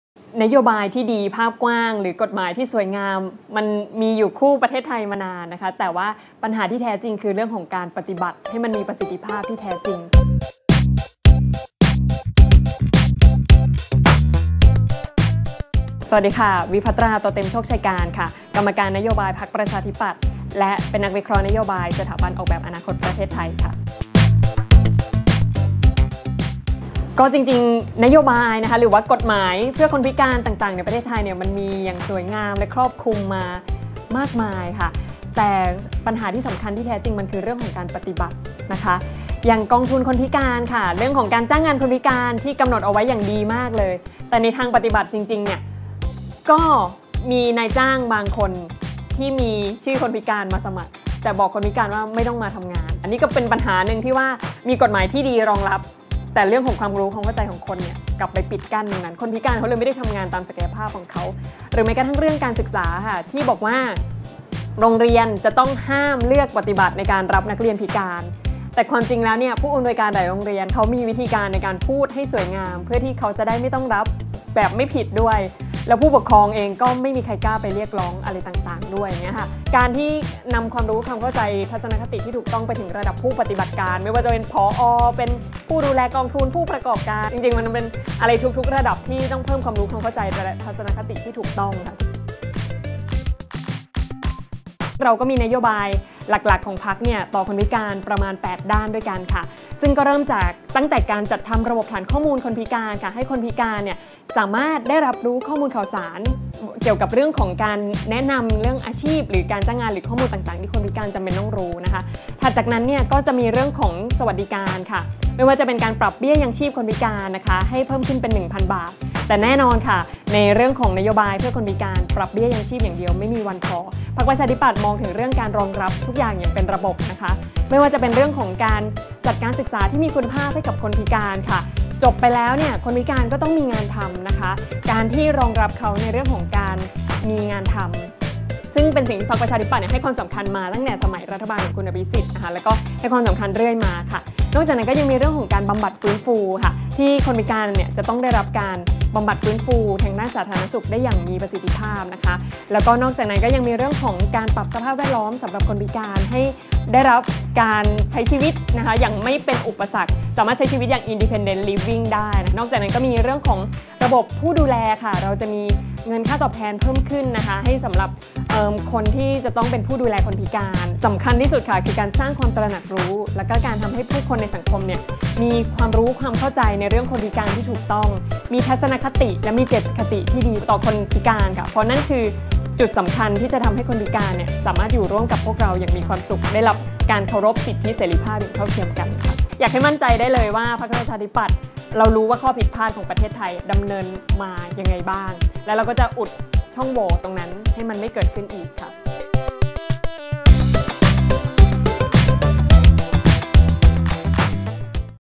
‘คนพิการอยู่ดี 8 ด้าน’ อ่านนโยบายที่ชูความเท่าเทียมจากพรรค ‘ประชาธิปัตย์’
"นโยบายที่มีภาพกว้างหรือกฎหมายที่สวยงาม มันมีอยู่คู่ประเทศไทยมานาน แต่ปัญหาที่แท้จริงเป็นเรื่องของการปฏิบัติ"
"อย่างกองทุนคนพิการ เรื่องของการจ้างงานคนพิการ ที่กำหนดเอาไว้อย่างดีมากเลย แต่ในทางปฏิบัติจริงๆ ก็มีนายจ้างบ้างคนที่มีชื่อคนพิการมาสมัครทำงาน แต่กลับบอกคนพิการว่า ไม่ต้องมาทำงาน นี่ก็เป็นปัญหาหนึ่งที่ว่า มีกฎหมายที่ดีรองรับ แต่เรื่องของความรู้ความเข้าใจของคน กลับไปปิดกั้นตรงนั้น คนพิการเลยไม่ได้ทำงานตามศักยภาพ"
คุยกับ วิพัตรา โตเต็มโชคชัยการ กรรมการนโยบาย พรรคประชาธิปัตย์ ถึงสิ่งที่เธอเห็นถึงปัญหาเกี่ยวกับคนพิการในสังคมไทย ทั้งปัญหาการนำนโยบายไปปฎิบัติ ทั้งเรื่องของความเข้าใจ ที่ยังรอการแก้ไข และพรรคจะทำอะไรในด้านคนพิการหากได้เป็นรัฐบาล
ติดตามซีรีย์สัมภาษณ์นโยบายคนพิการจากหลายพรรคการเมืองในช่วงใกล้เลือกตั้งได้เร็วๆ นี้ที่ ThisAble.me เท่านั้น